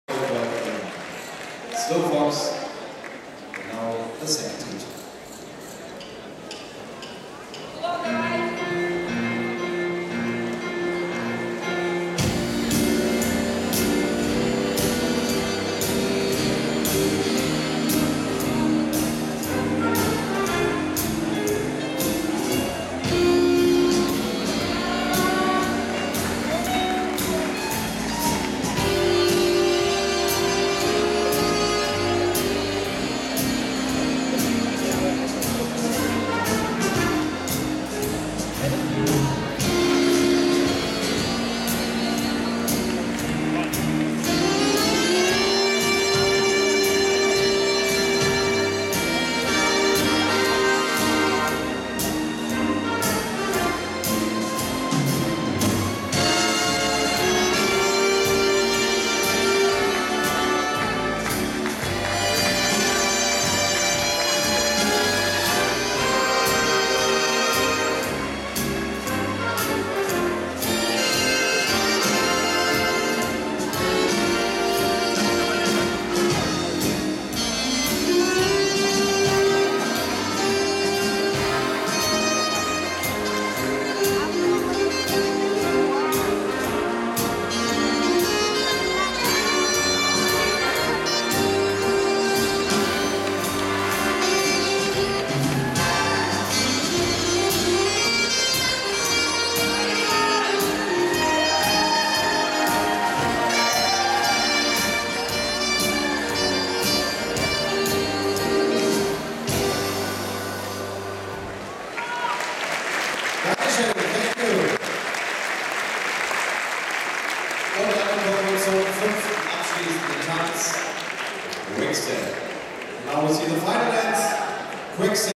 GOC 2009 Rising Star Standard - Andres Liiv & Kristin Vaha SF
Andres Liiv & Kristin Vaha dancing slow foxtort in the semifinal in Amateur Rising Star in Stuttgart.
21.08.09